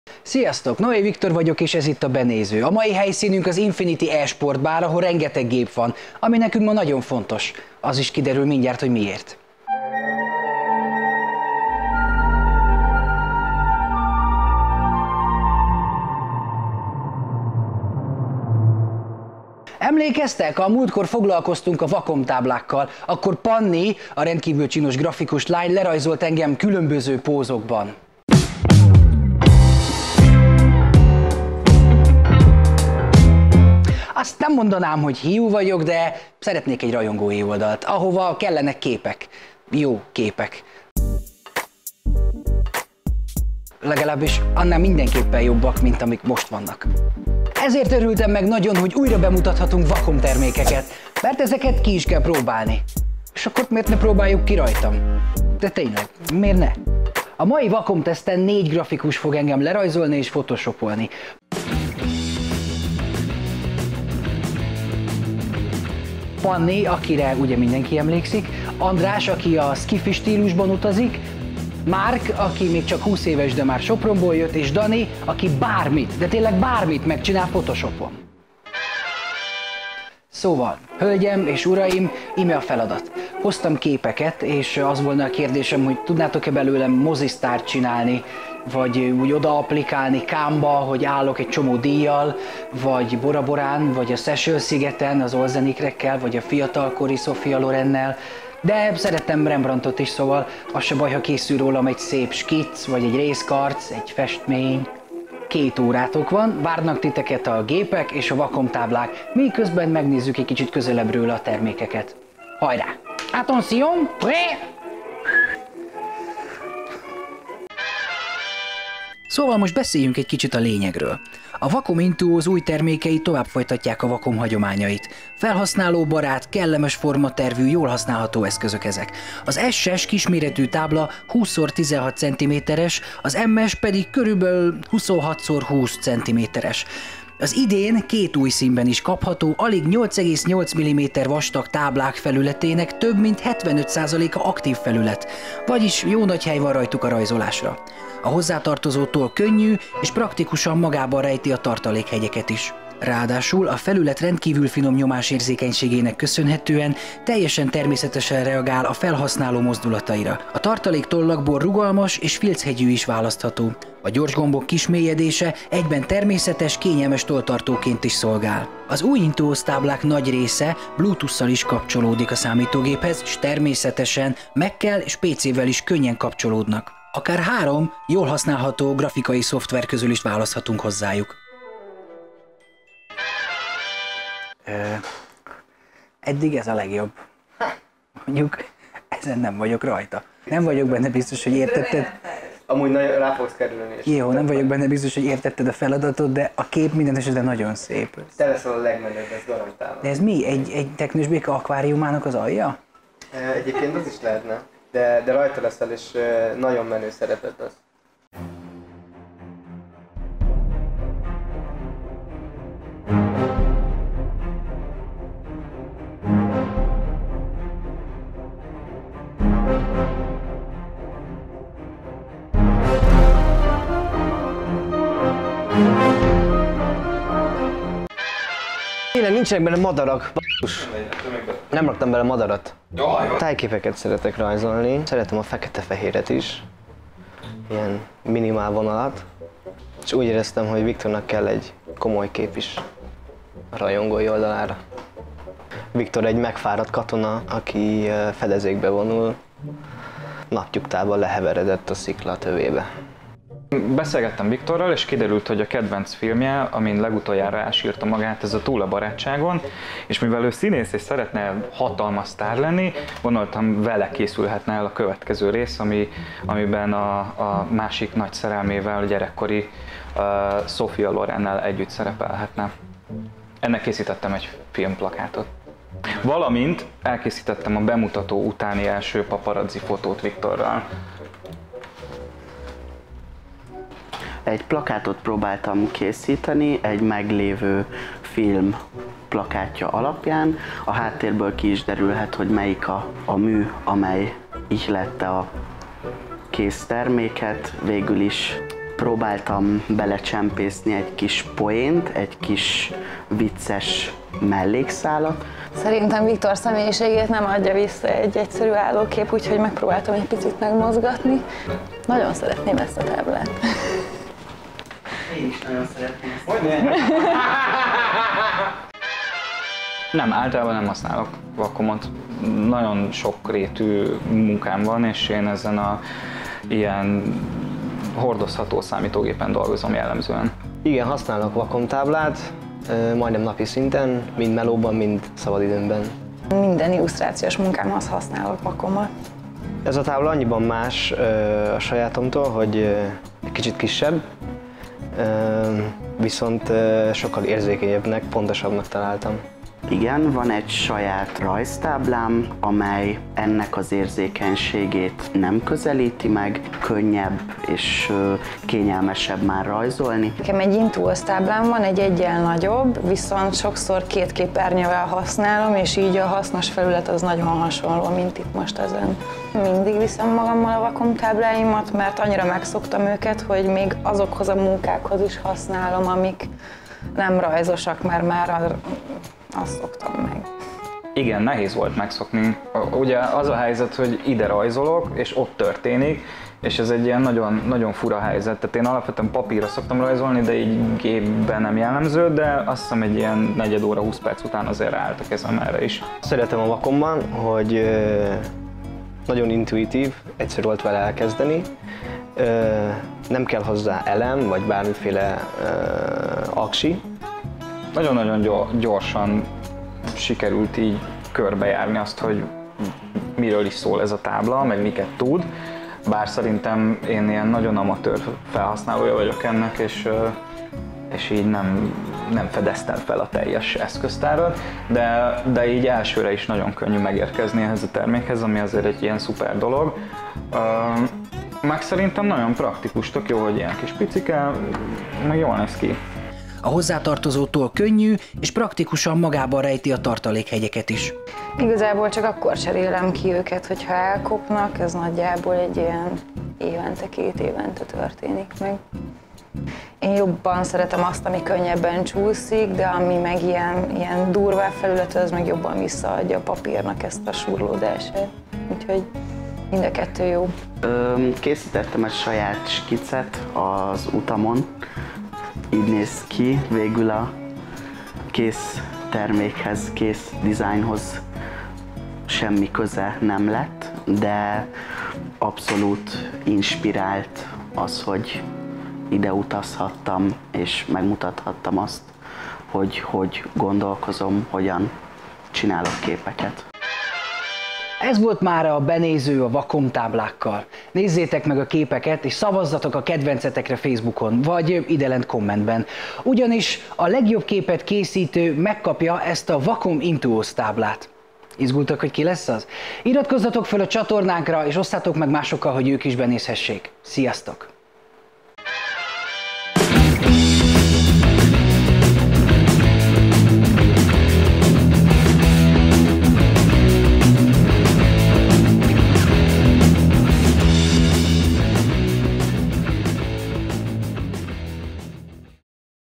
Rajzold újra Viktort - A nagy Wacom teszt
Az új Wacom Intuos táblák világába kalandozunk el, ahol grafikus vendégeink megpróbálják egy kicsit feldobni Viktor rajongói oldalát.

Neked melyik mű tetszett a legjobban?